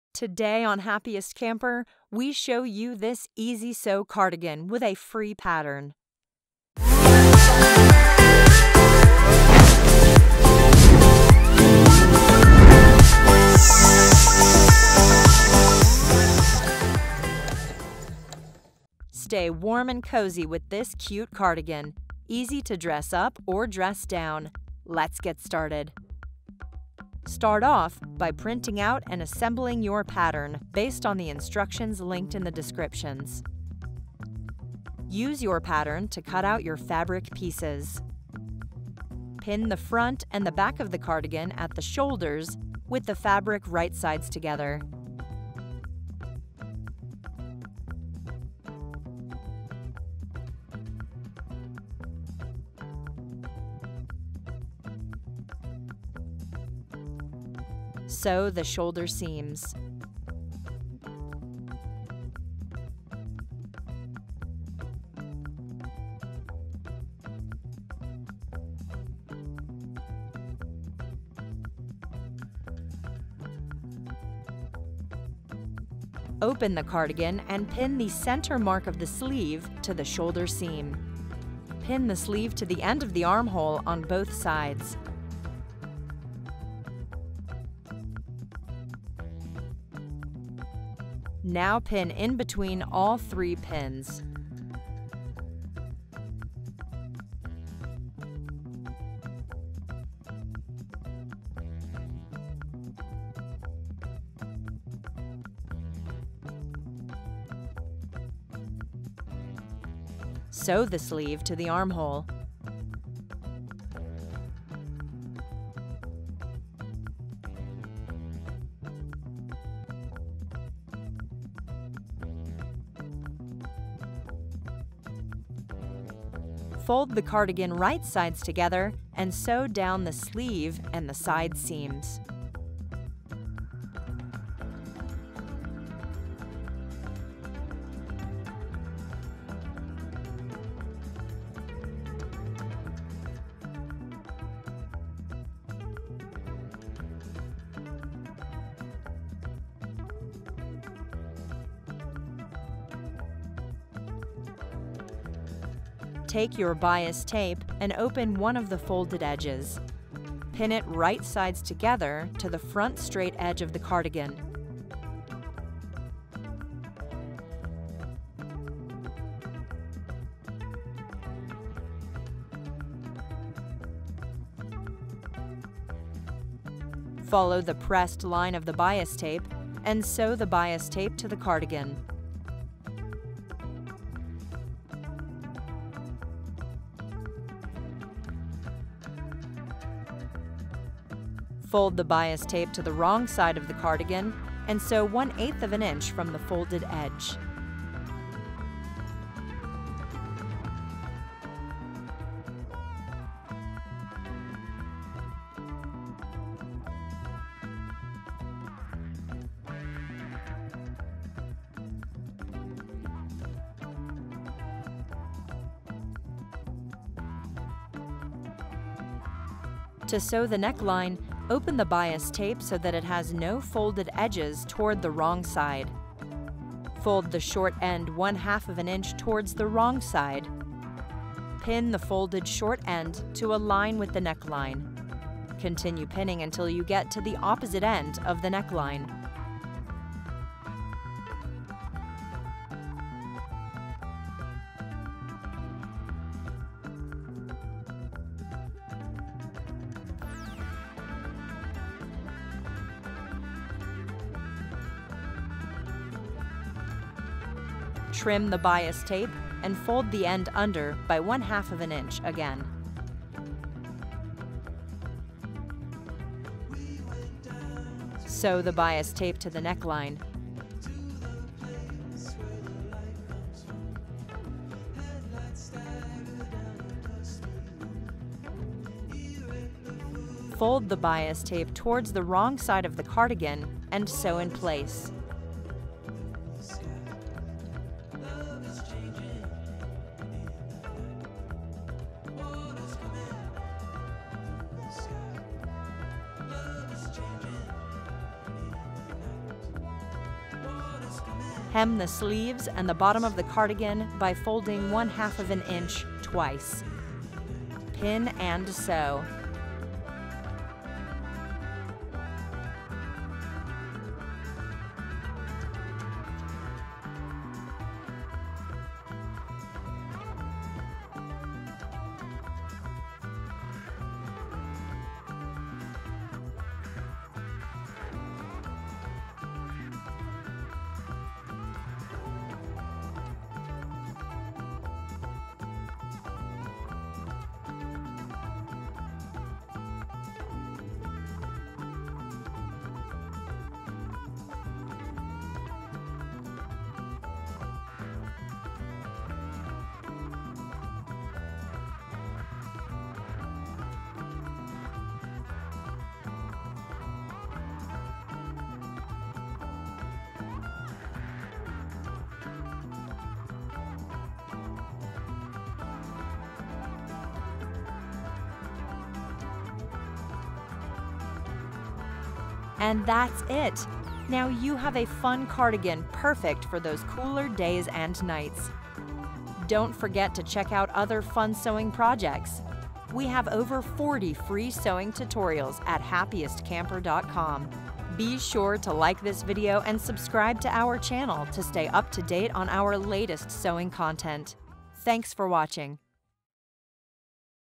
Easy Sew Cardigan With Free Pattern
Stay warm and cozy with this cute easy sew cardigan with free pattern! I love a soft and cozy layer for those chilly days and nights. Pair this cardigan with your favorite dress or keep it casual with jeans. I love this easy sewing project with a free pattern to make your of DIY Cardigan. This simple sewing tutorial will have you making your own in no time! Full step by step tutorial with photos + free downloadable pattern @